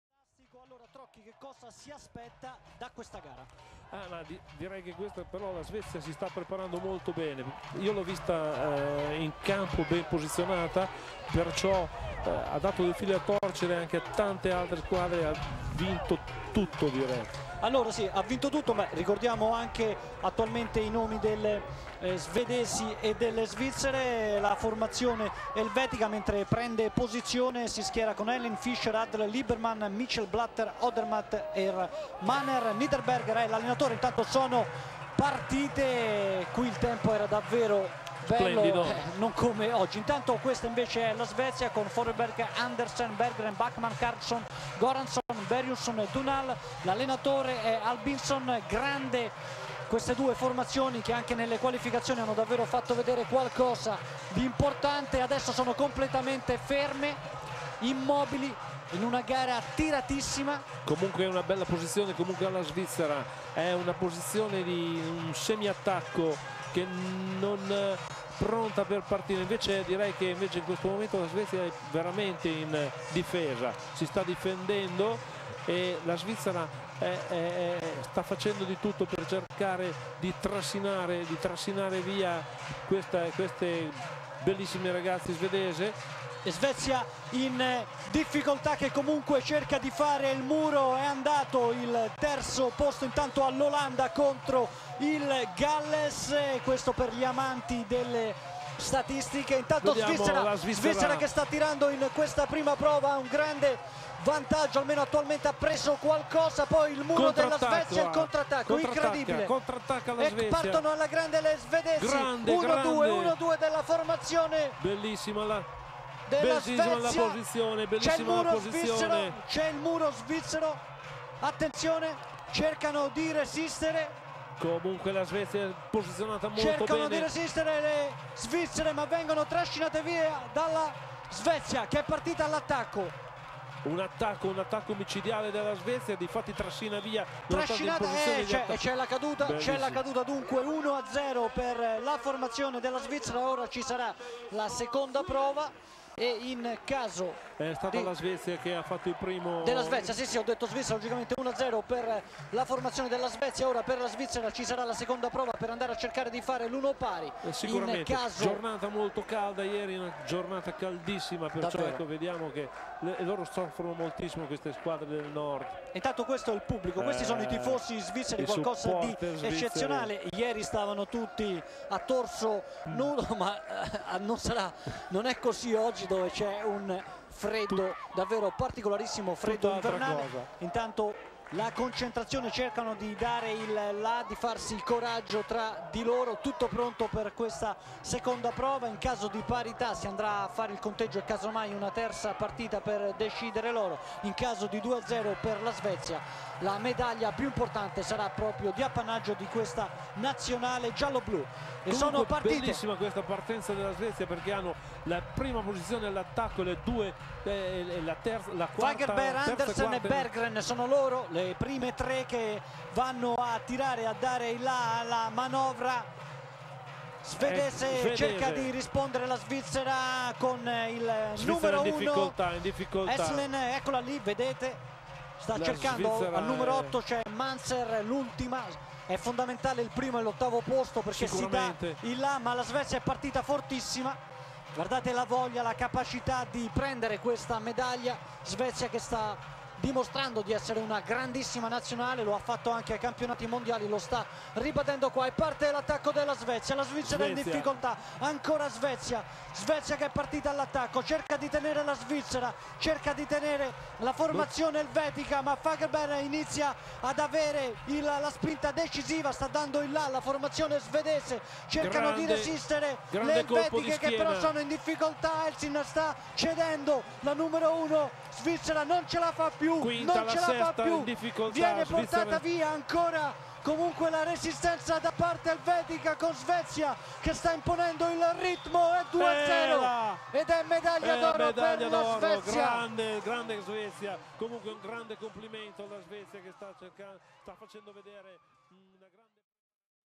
Tug of War European Championships 2005 Women 560kg
European Championships 2005 - Women 560kg Weightclass Gold Medal Match between Switzerland and Sweden

Tug of War is a global, team, power sport.
Tug of War International Federation is an officially recognized by IOC body that governs this sport.